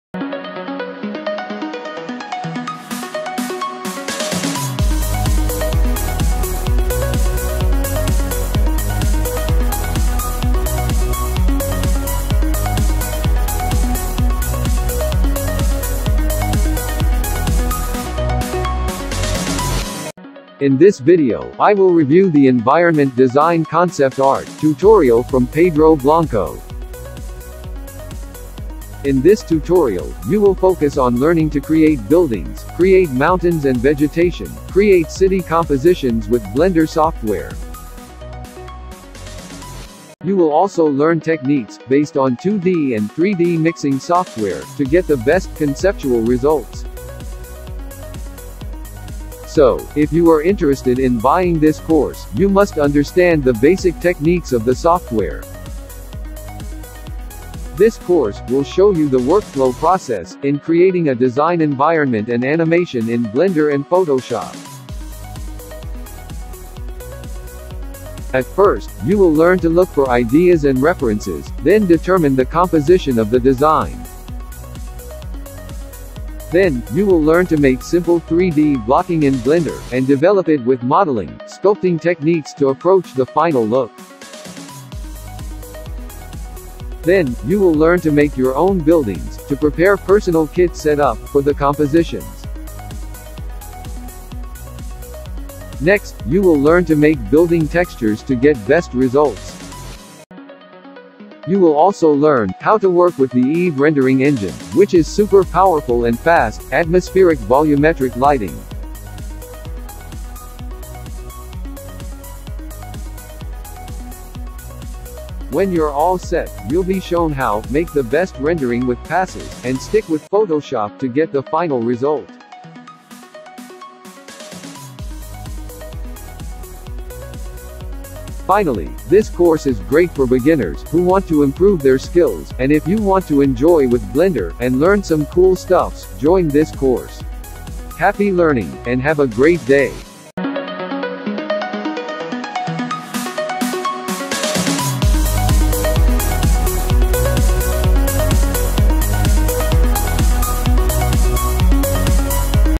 Sci-fi Environment design concept art COURSE REVIEW
This is a review of the Blender and photoshop concept art environment design course currently available through Wingfox. It a set of courses created by Pedro Blanco a freelance concept artist.
It's focused on creating building, mountain and vegetation with  Blender, Mixing with 2d software photoshop to approach best result.